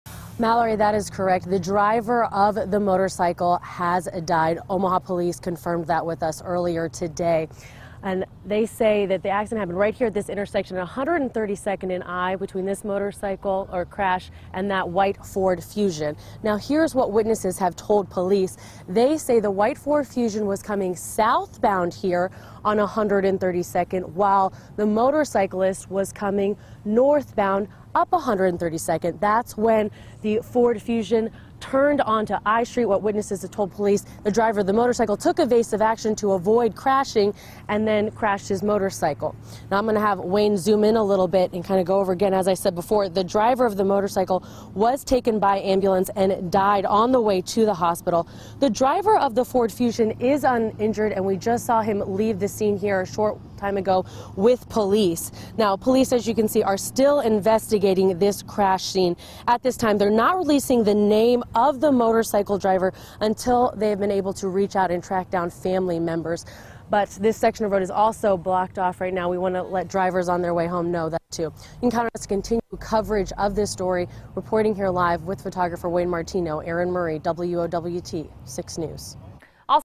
Motorcyclist Killed In Crash
Crash Near 132nd Street & I Street